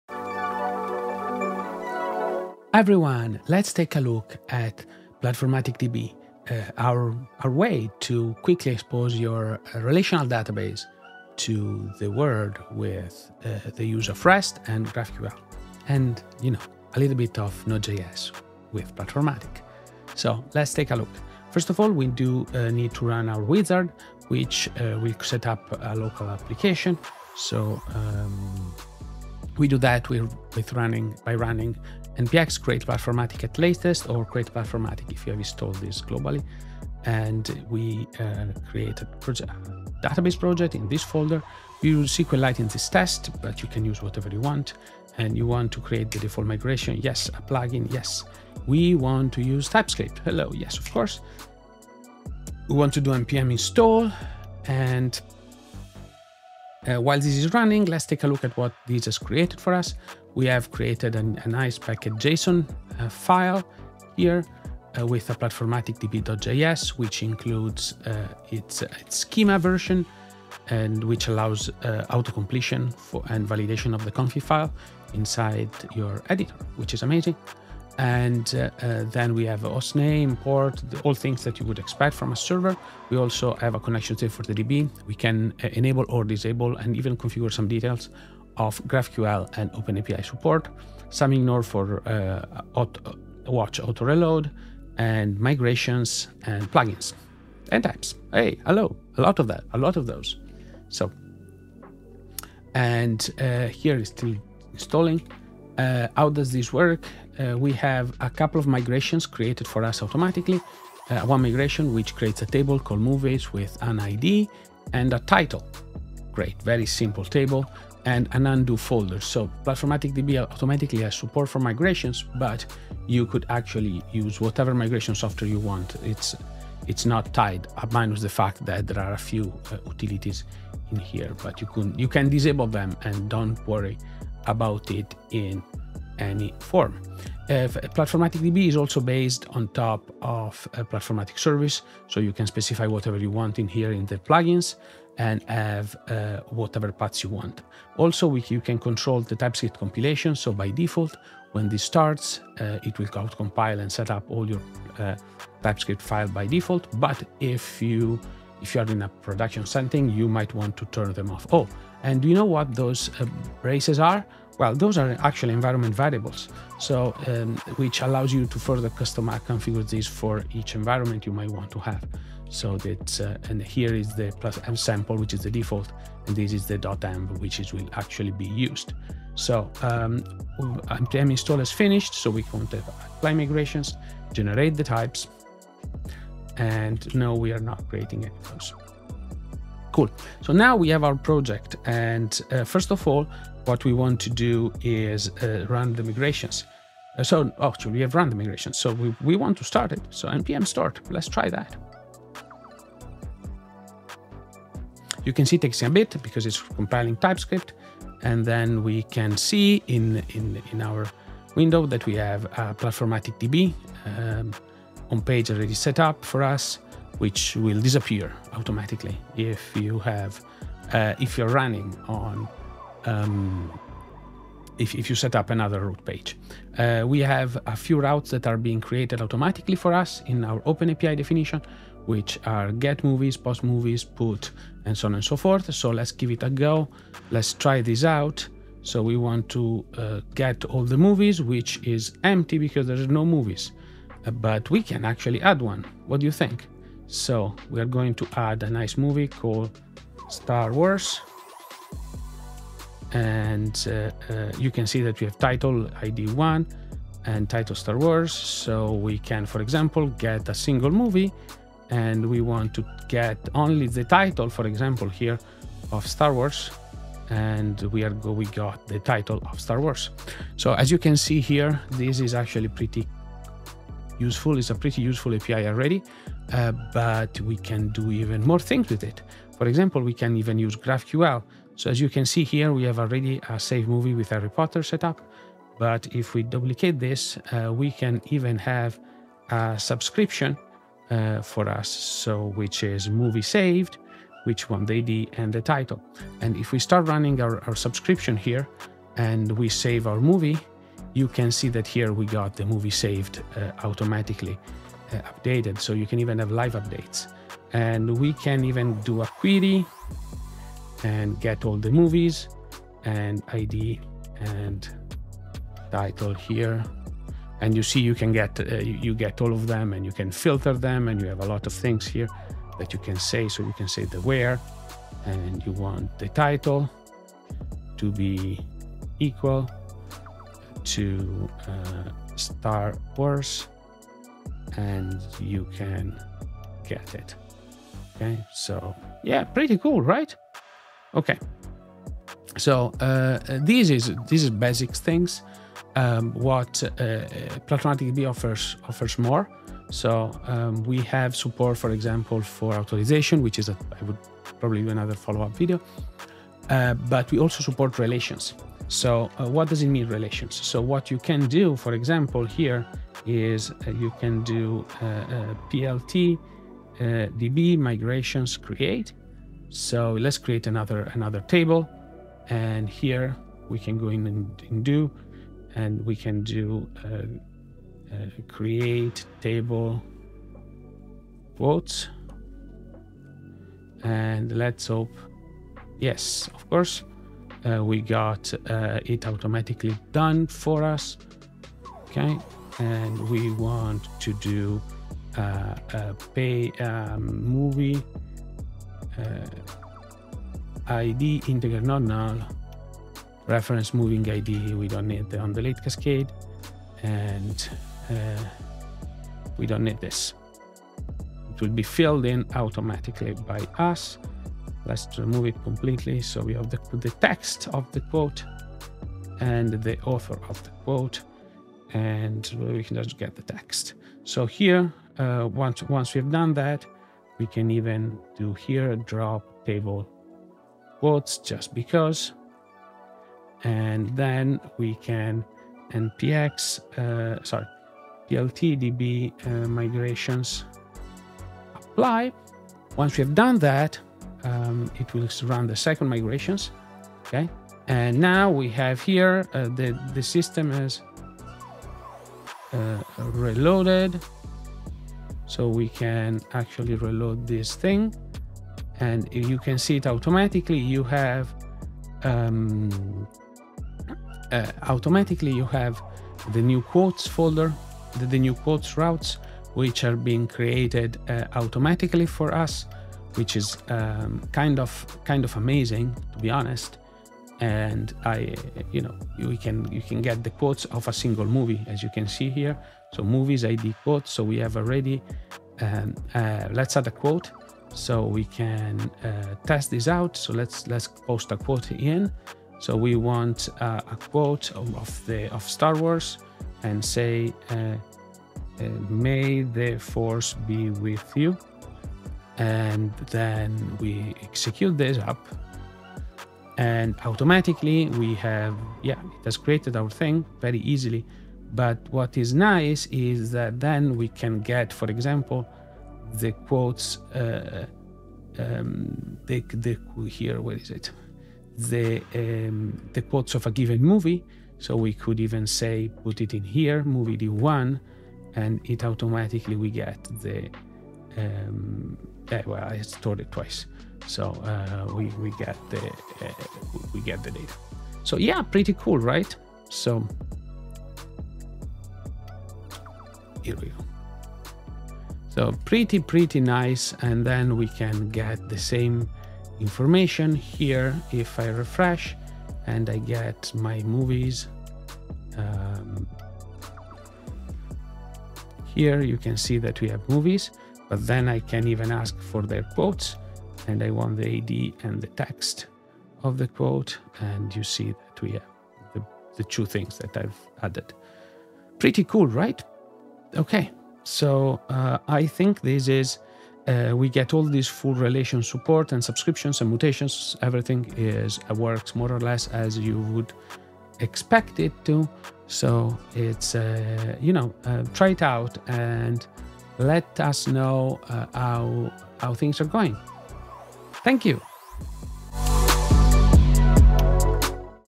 Platformatic DB- How does it work?
With Platformatic DB, you can turn any database into a REST and GraphQL endpoint with complete flexibility via Fastify plugins.

Tired of creating Create-Read-Update-Delete (CRUD) APIs?  Platformatic DB allows you to create both OpenAPI and GraphQL schemas from your database, without having to write a single line of code. Customize your APIs to their fullest extent with Node.js and Fastify plugins.

In this video, we do a quick demo to show how DB works.